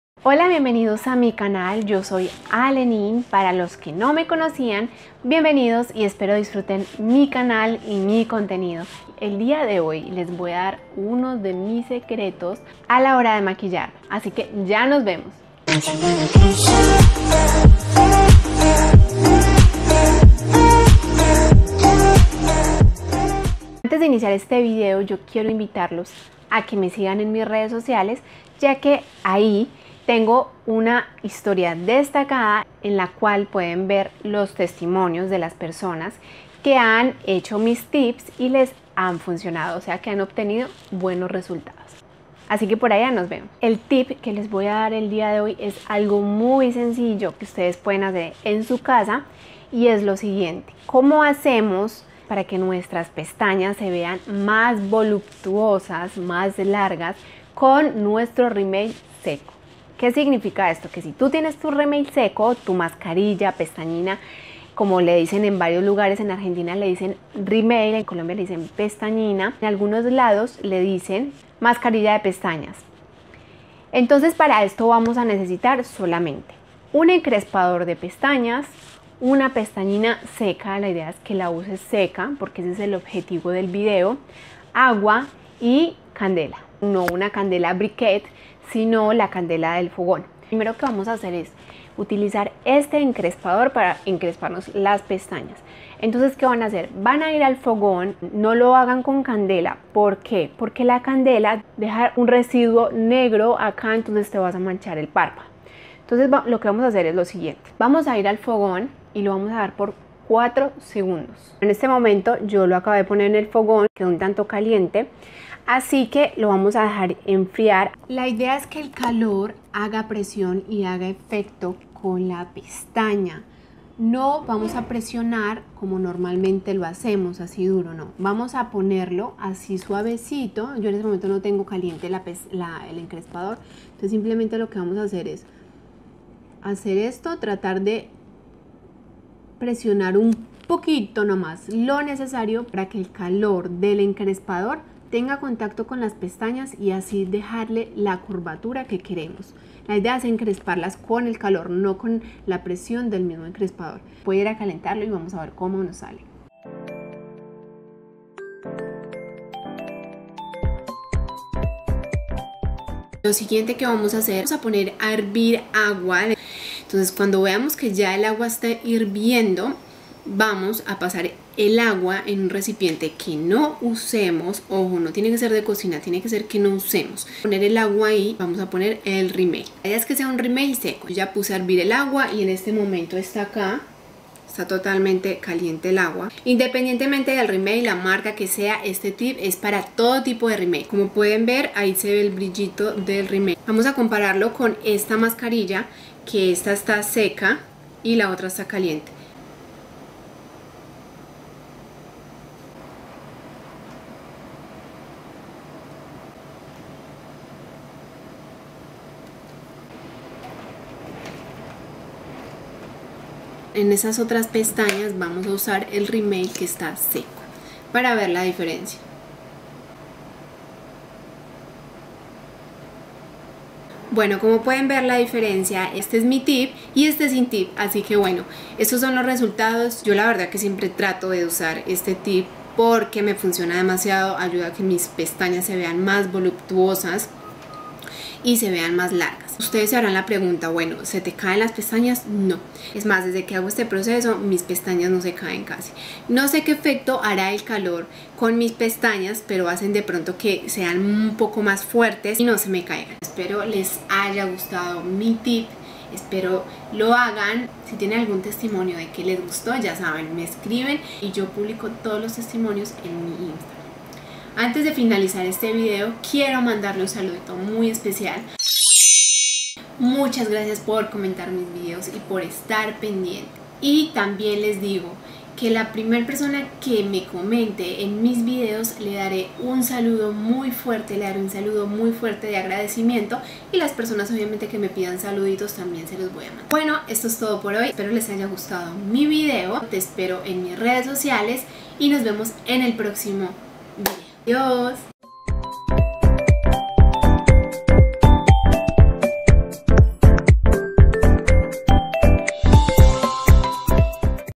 Hago mis pestañas a calor, con rimmel o pestañina seca, Alenin Mejia
Hola amigos de youtube, hoy les traigo un video que hace rato se los quería mostrar, espero les guste

No es publicidad pagada, es un video voluntario.